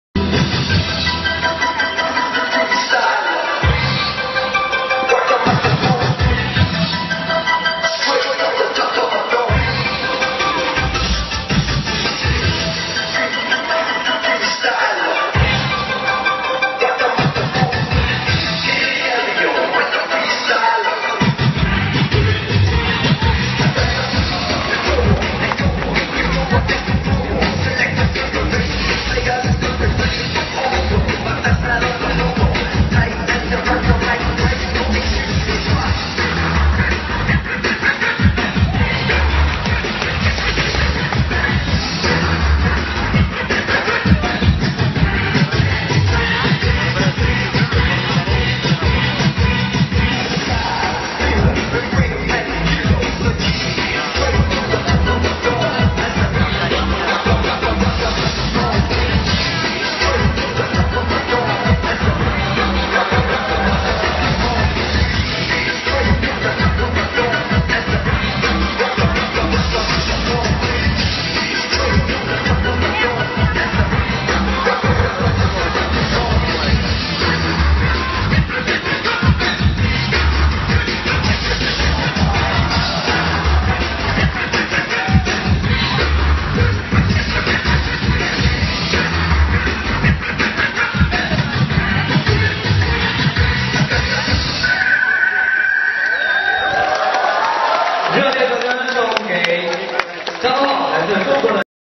Asian Kid With Mad Roller Blading Skills
Wonder how much time he spents practicing this, it's awesome!